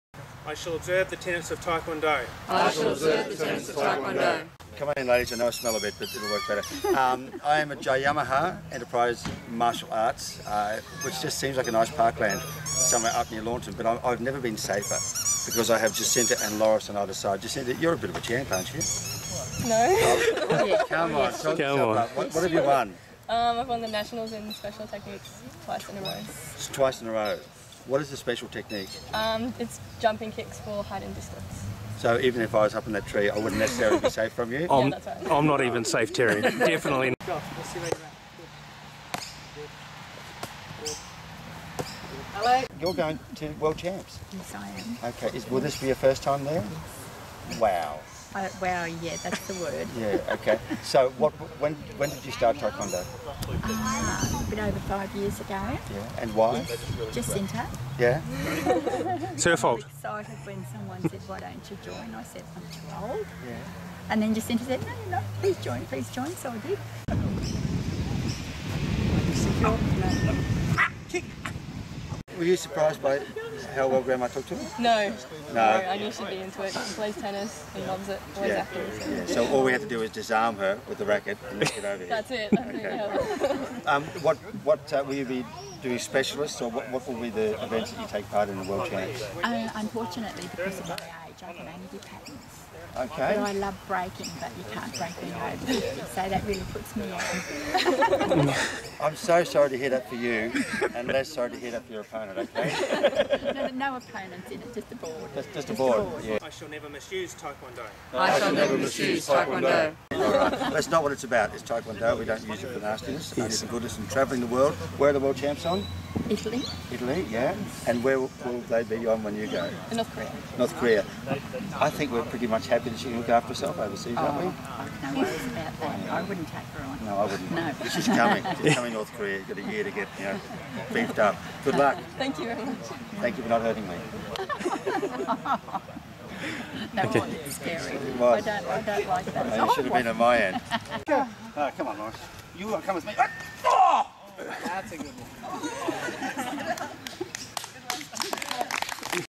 DB TKD Jesinta and Loris Jayahama Enterprise Martial Arts
DAD BODS  Meet the Grandmother Loris / Granddaughter Jesinta Duo headed for the next two Tae Kwon Do World Champs!